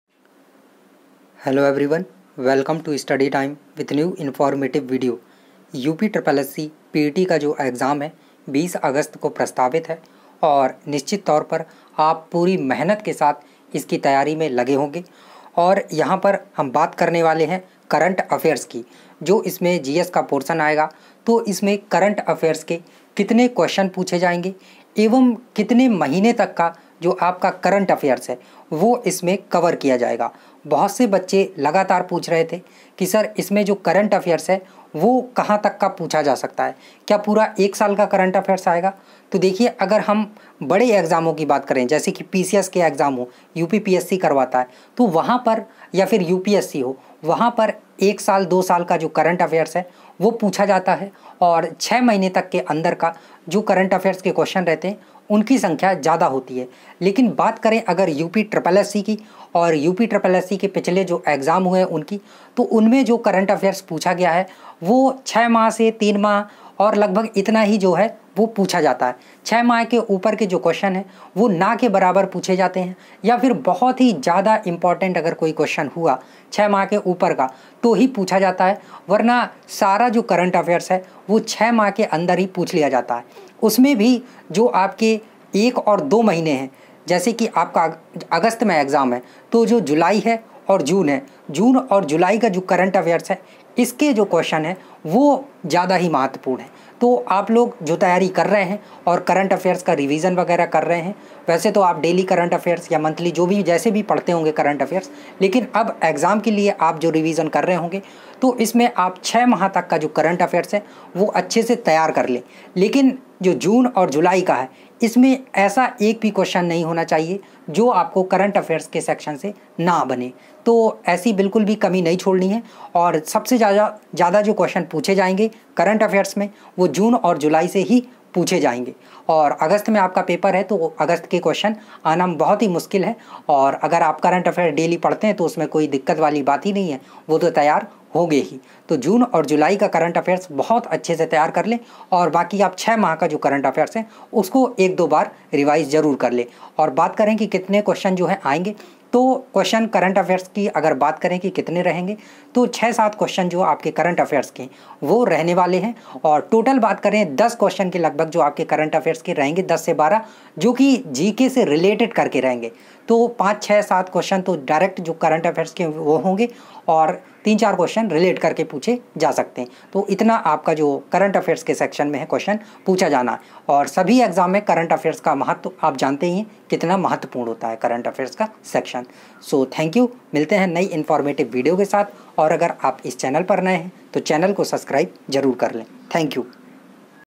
UPSSSC PET Exam में कितने महीने का करंट अफेयर्स आएगा ? | UPSSSC PET Latest News | studytime
Ask Your Questions In Comment Box.

Join Telegram Group For More Update Releted To Government Jobs. And Also Get Study Material.

Topic Cover :-
1. UPSSSC PET Current Affairs
2. UPSSSC Current Affairs
3. UPSSSC Latest News
4. PET Exam News